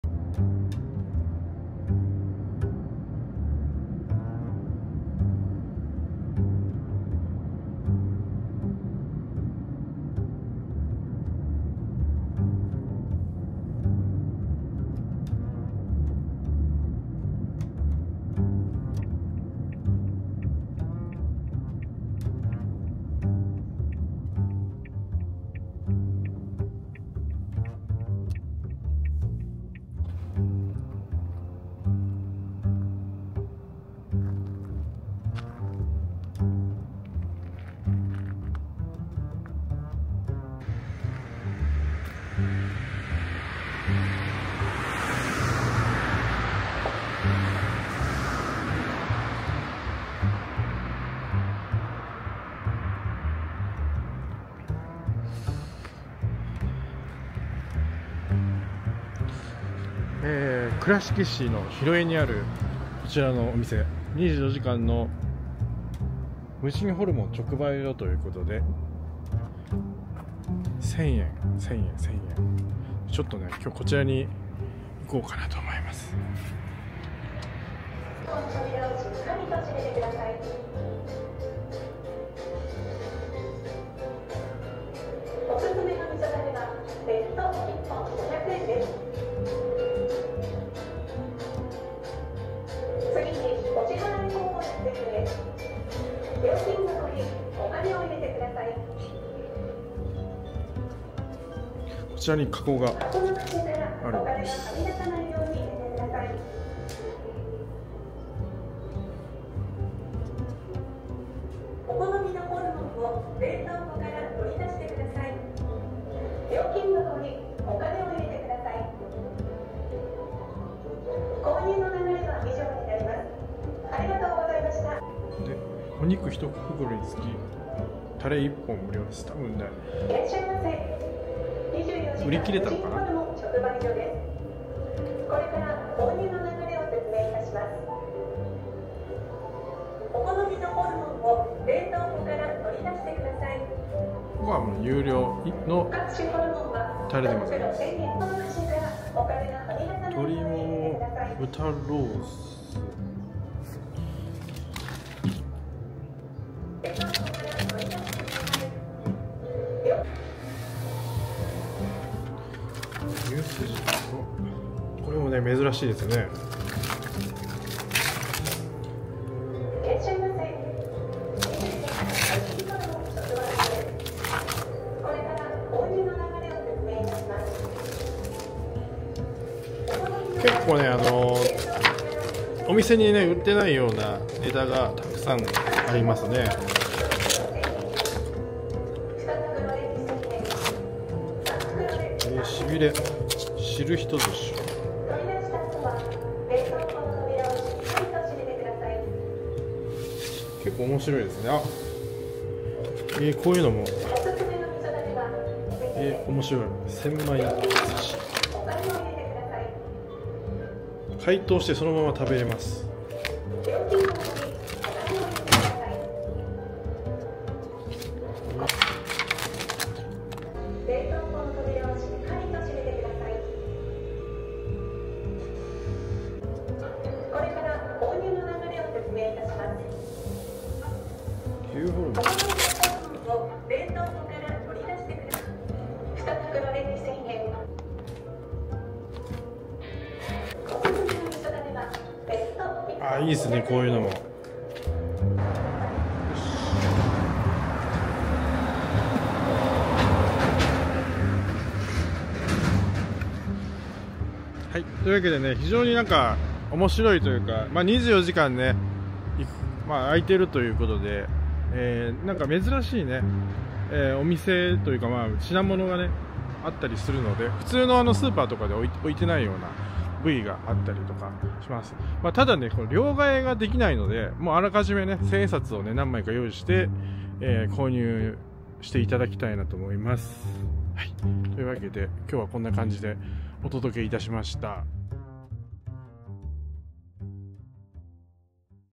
【 岡山県倉敷市にできた無人の焼肉・ホルモンの販売所へ潜入】岡山では入手困難な焼肉用のネタ！24時間営業の焼肉専門！
24h 無人ホルモン直売所　倉敷水島店
倉敷市広江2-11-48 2号室

再生リスト